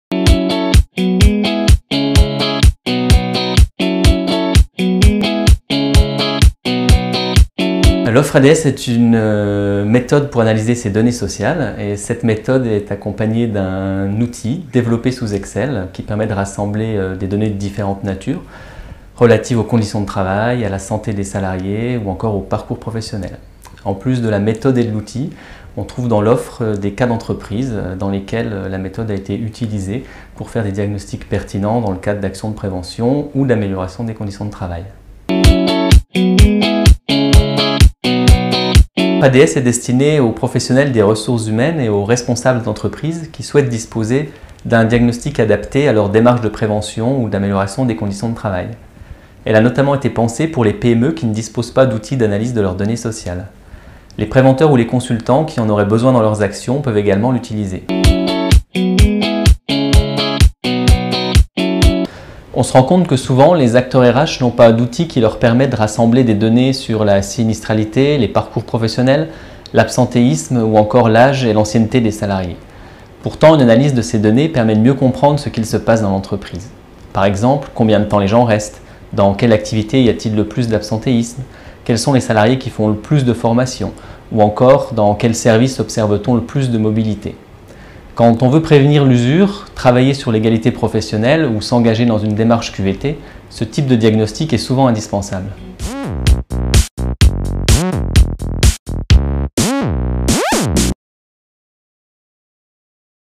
Qu'est-ce que le kit "Analyser ses données sociales" (ADS) de l'Anact ?
Vous êtes professionnel(le) des ressources humaines ou encore responsable d'entreprise ? Le kit "Analyser ses données sociales" a été conçu pour vous outiller dans votre démarche d'amélioration des conditions de travail.